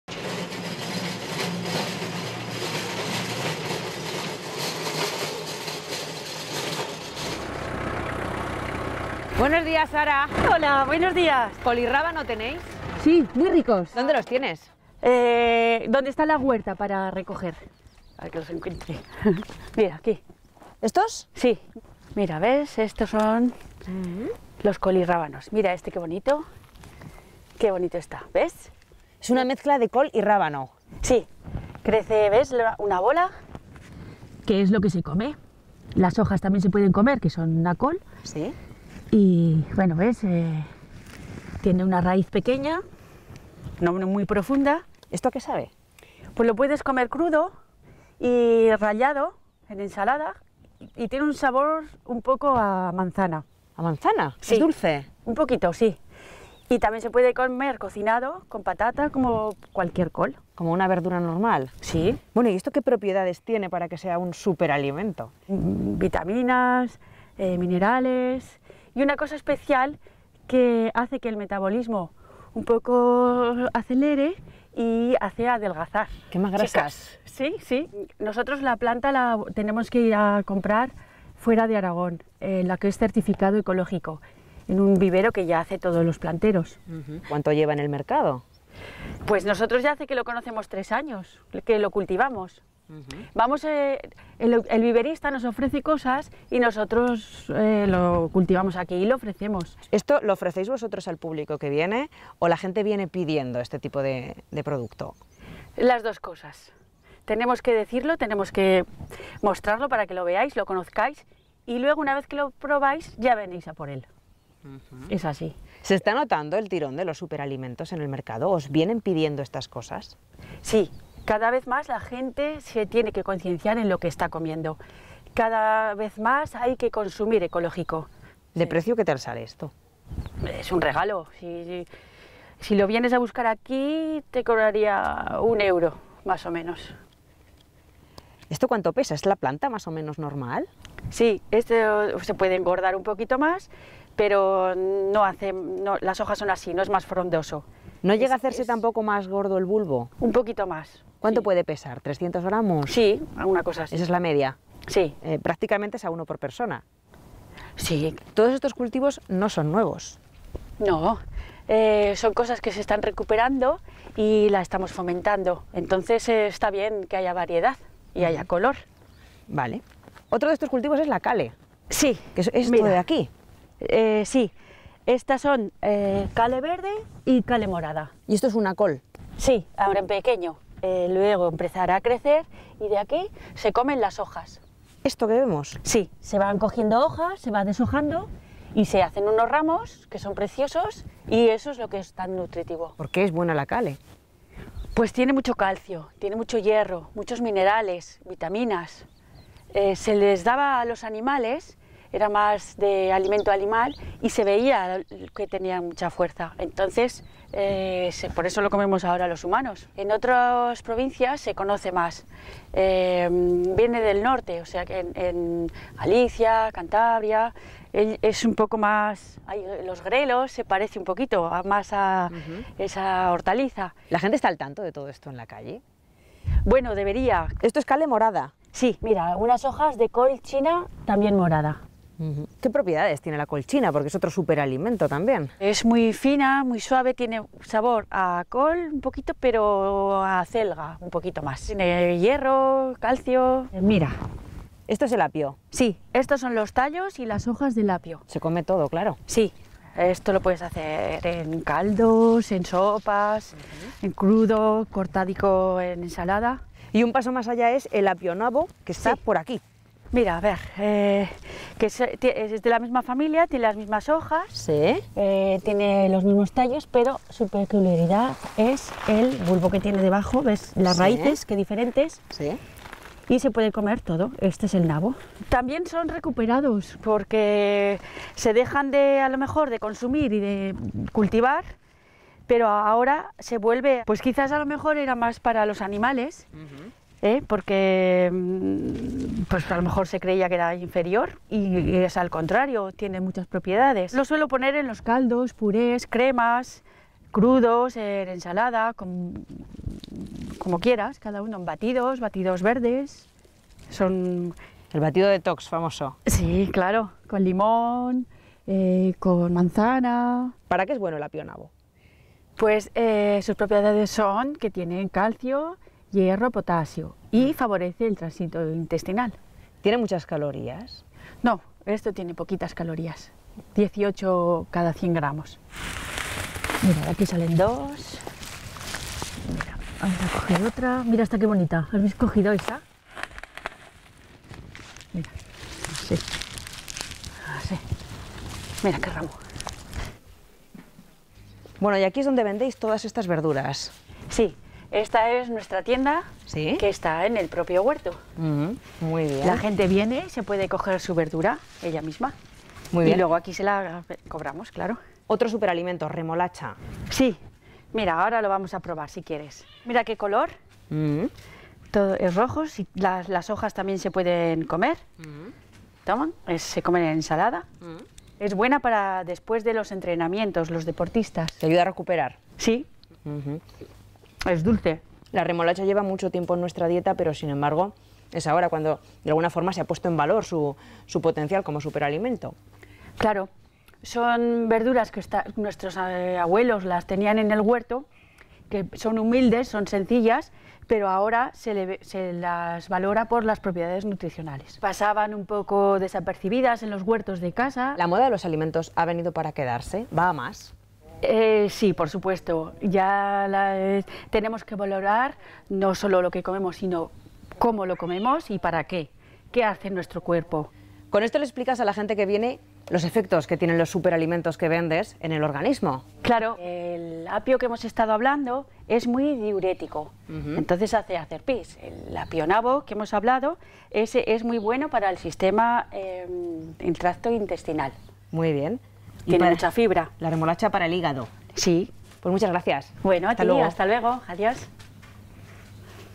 UNIDAD MOVIL. SUPERALIMENTOS ARAGONESES 5
La huerta de Zaragoza es rica en superalimentos. Unos han llegado nuevos y otros se han recuperado. Es el caso del colirábano, vegetal mitad col y mitad rábano que crece sobre todo en invierno y que hace tiempo ya se utilizaba como alimento para los animales. Con la moda del running, la remolacha se ha puesto en valor por su alto valor para la recuperación física tras el deporte. Apionabo o kole también se están abriendo paso en nuestra despensa.